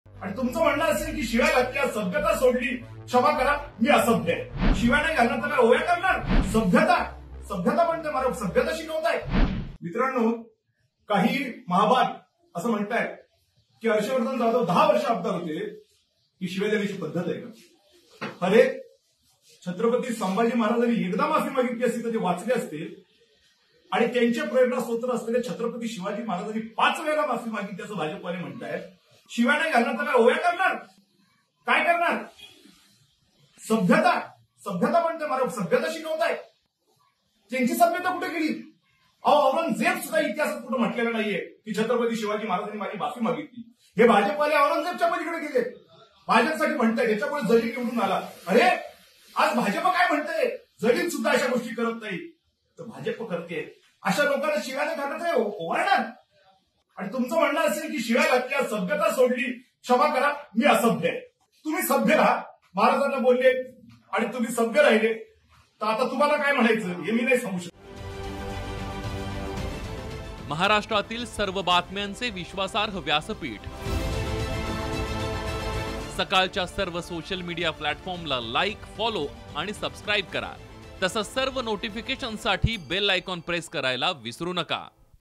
Harshvardhan Jadhav on BJP | हर्षवर्धन जाधवांचा पुन्हा भाजपावर निशाणा | Politics | Sakal
राज्यपाल भगतसिंग कोश्यारींनी शिवाजी महाराजांवर आक्षेपार्ह टीका केल्यानंतर राज्यभरातून टीकेची झोड उठत आहे. यातच माजी आमदार हर्षवर्धन जाधव यांनी भाजपावर निशाणा साधला होता. त्यानंतर भाजपाकडूनही त्यांच्या अपशब्दावर आक्षेप घेतला होता.  यावर आता पुन्हा हर्षवर्धन जाधवांनी थेट चहा टपरीवरुन हल्लाबोल केला आहे.